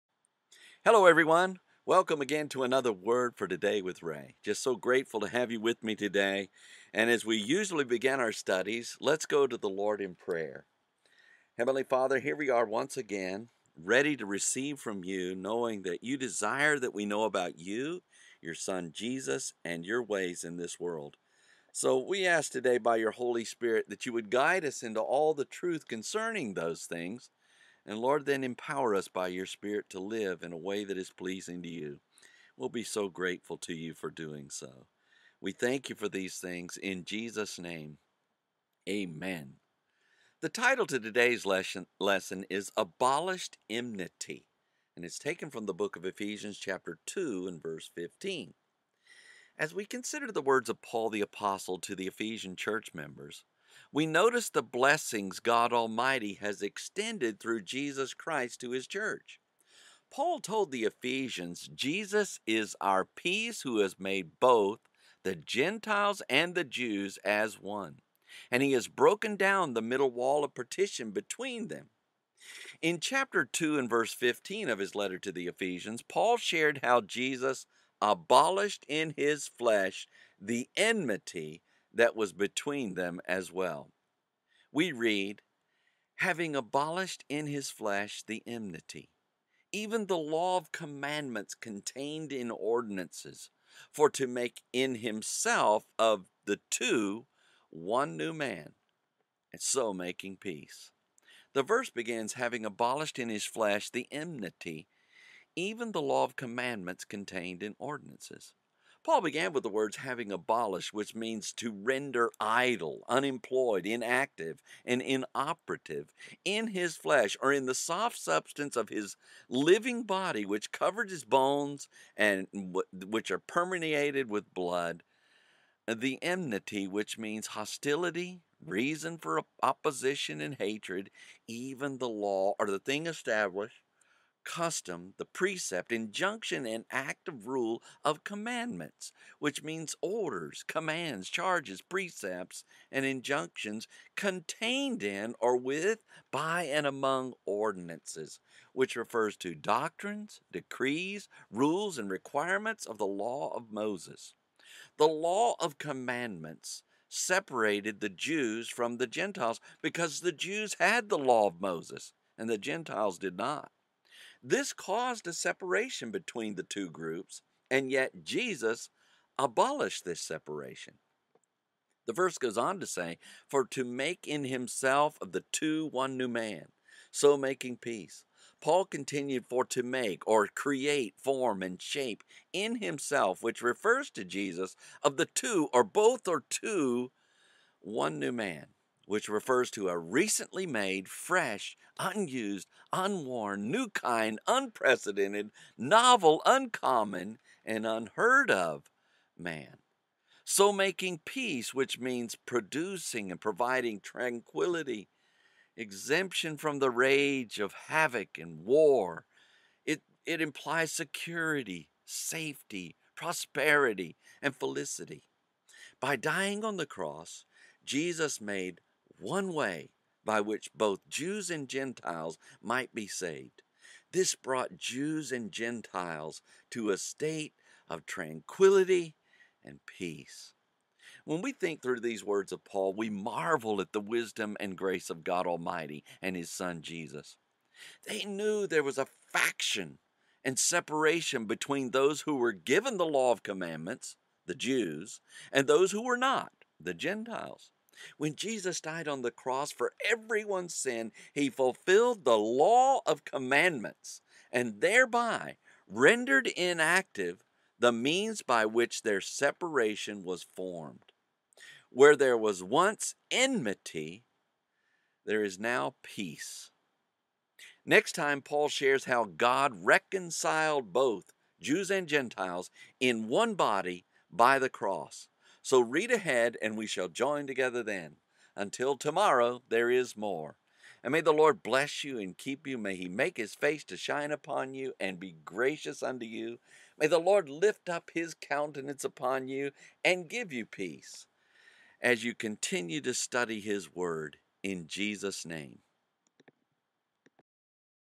Abolished Enmity - Ephesians 2:15 (made with Spreaker)
Verse by verse study through the book of Ephesians Chapter Two and Verse Fifteen